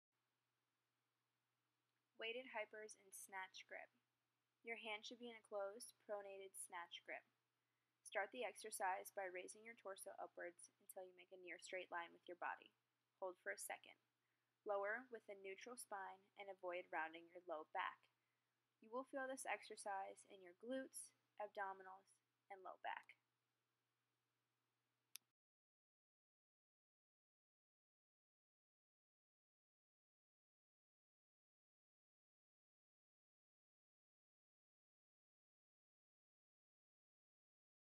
Weighted Hypers - snatch grip
- Your hands should be in a closed, pronated snatch grip.
- Start the exercise by raising your torso upwards until you make a near straight line with your body.
- Hold for a second.
- Lower with a neutral spine and avoid rounding your low back.
- You will feel this exercise in your glutes, abdominals, low back.